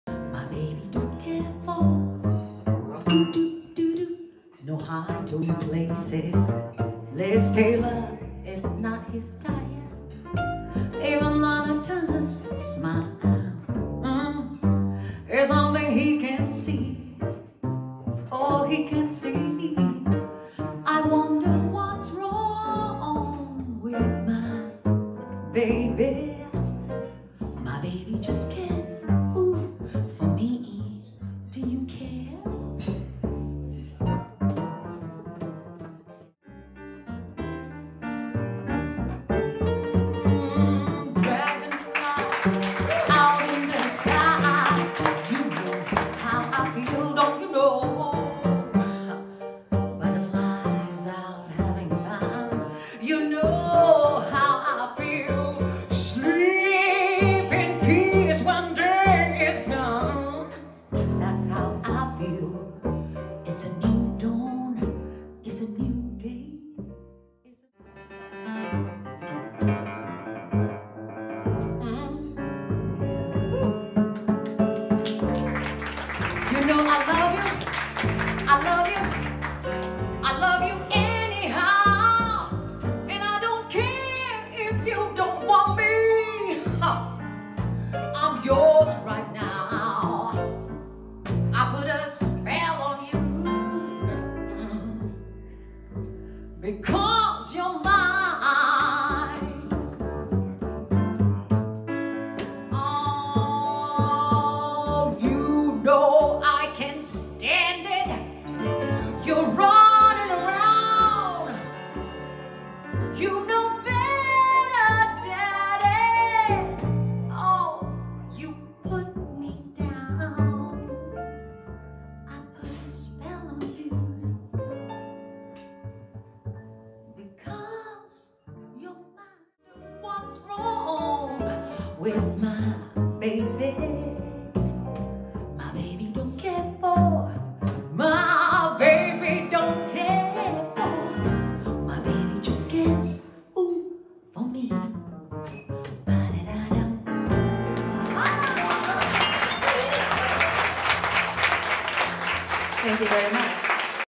LADIES OF JAZZ, SOUL AND BLUES - NINA SIMONE
Brisbane  Jazz Club
Highlights

Eunice Kathleen Waymon had to change her name to Nina Simone to hide from her religious family members who deemed cocktail music as "the devil's music" and looked at her work as "unholy employment "